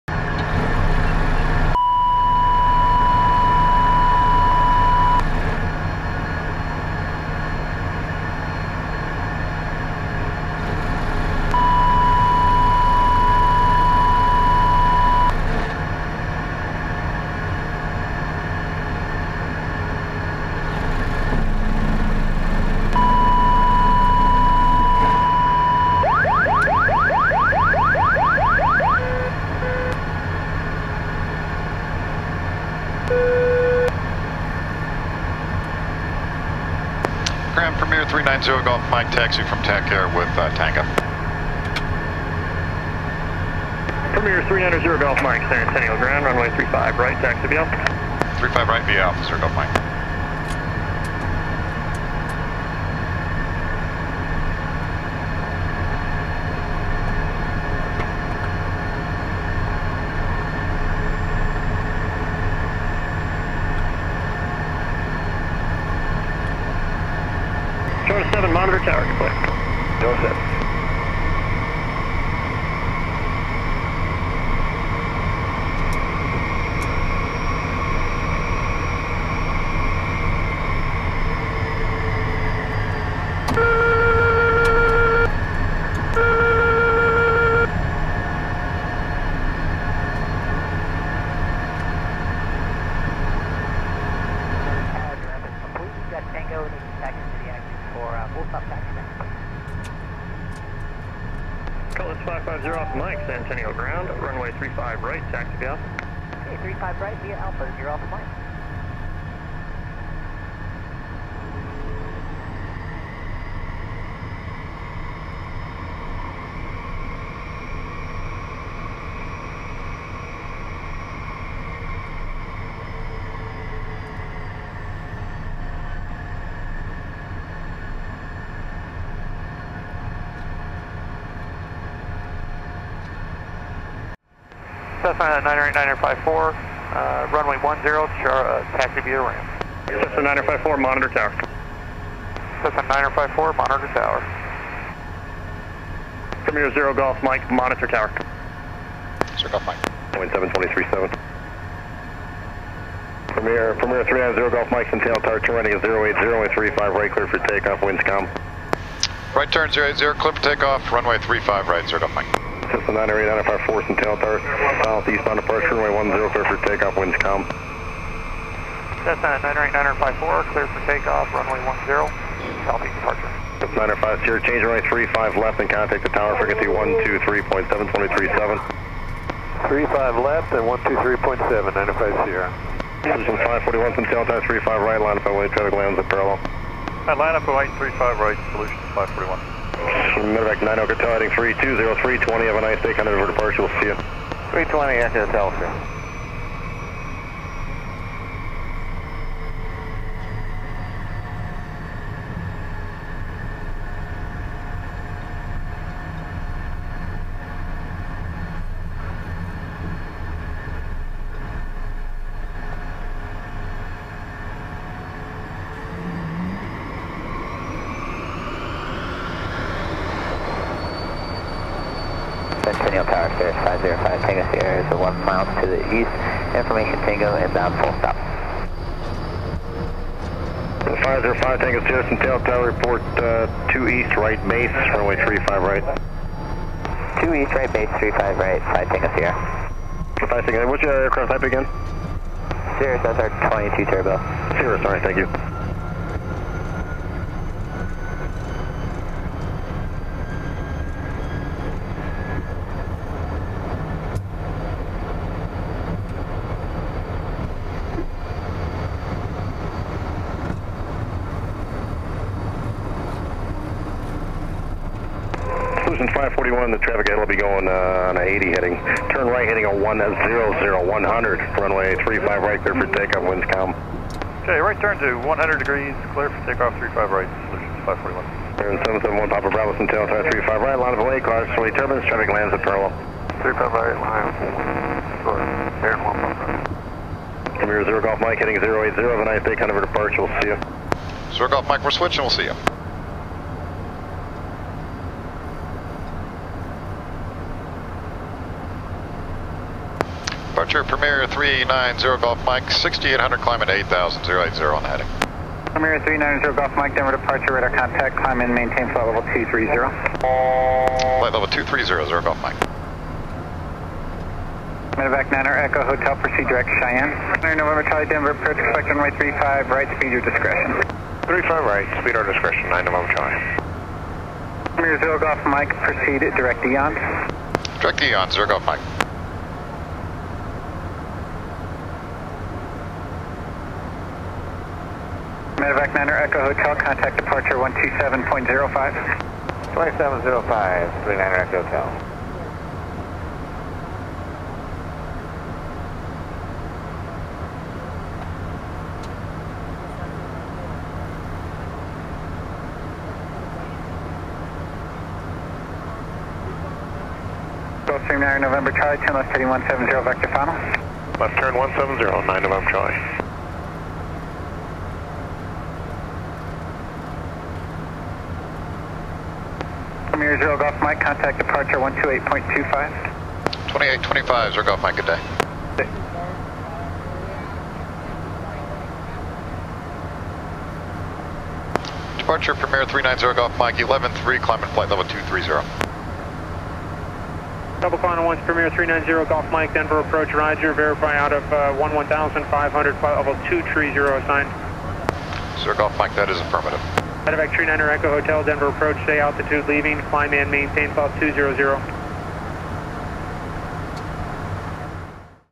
Takeoff from Centennial Airport Denver in a private jet
EEONS departure out of Denver Centennial airport in a Beechcraft Premier jet.